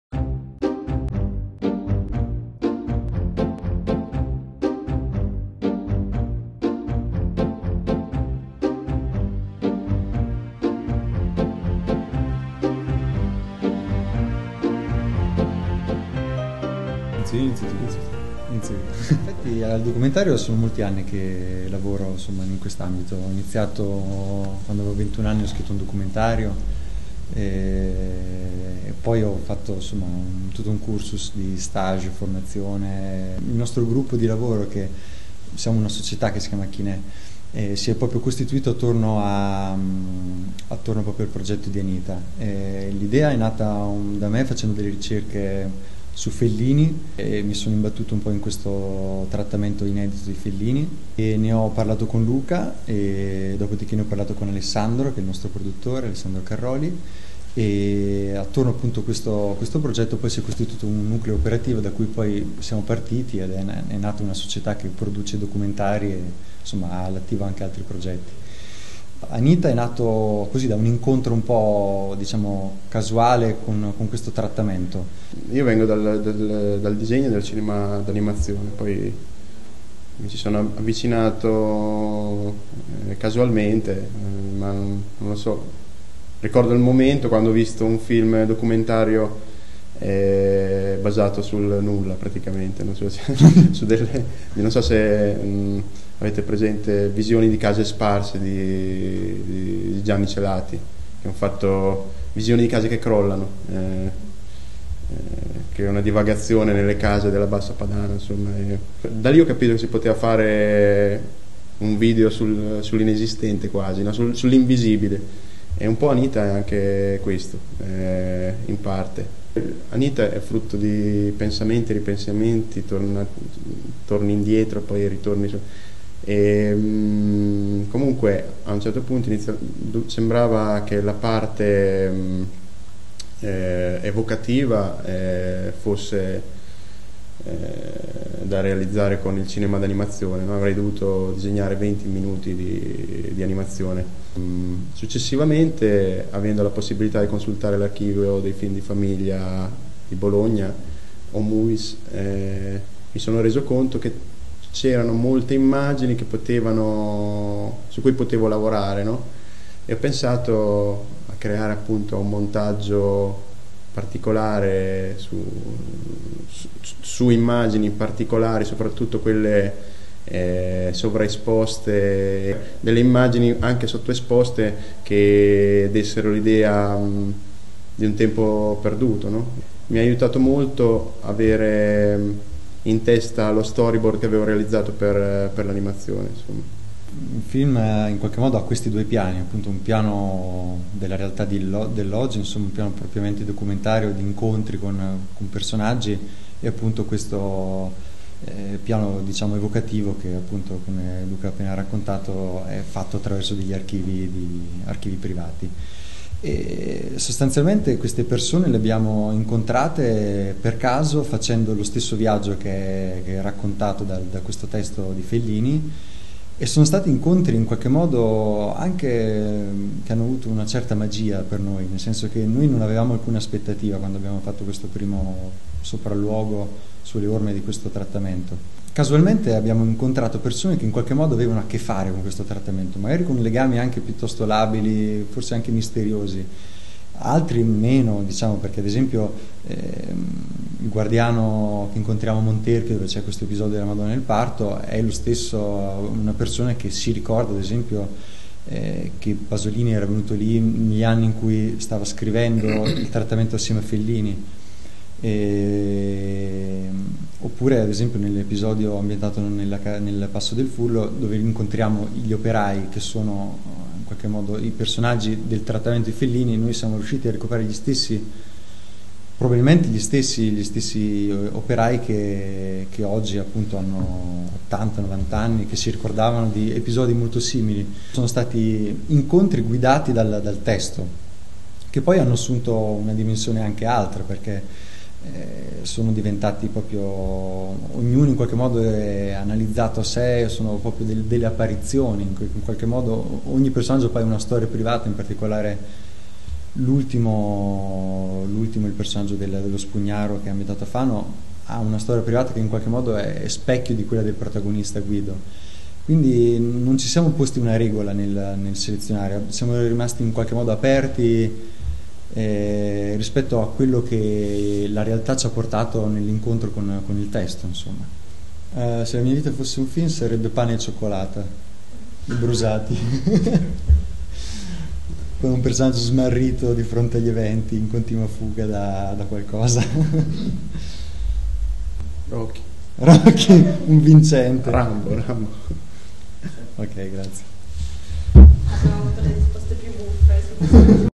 TFF OFF 2012 - Intervista a Luca Magi e Antonio Bigini
Luca Magi e Antonio Bigini, rispettivamente regista e sceneggiatore di Anita, al TFF OFF.